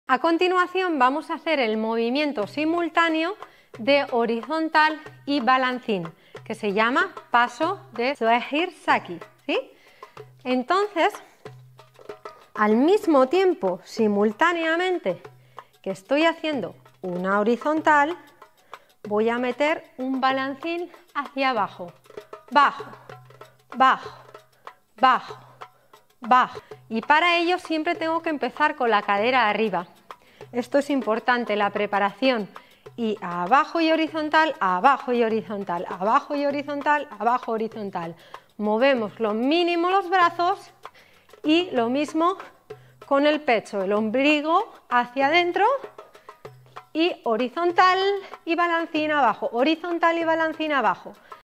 ¿Cuál es el resultado de unir dos movimientos simultáneamente?
✨Te hablo del paso mítico de Soheir Zaki✨
Es muy importante aprender la técnica desde la base para alcanzar movimientos más complejos como este. ➡️(Pincha Aquí) para descubrir mi curso online de nivel Intermedio)